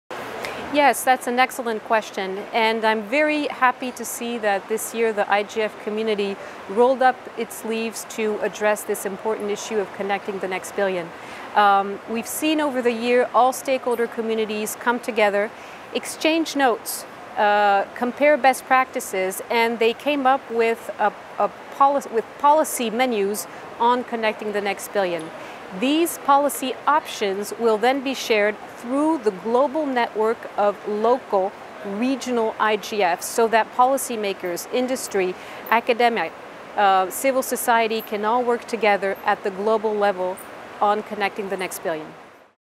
Global IGF 2015: Constance Bommelaer on Connecting Billions More
Constance Bommelaer, senior director of global Internet policy at the Internet Society, shares her answer to the question, “What steps are being taken to connect the ‘last billion’ people, those most difficult to reach and serve?” Recorded at the Global Internet Governance Forum 2015 in João Pessoa, Brazil, by the Imagining the Internet Center, an initiative of Elon University, North Carolina, USA.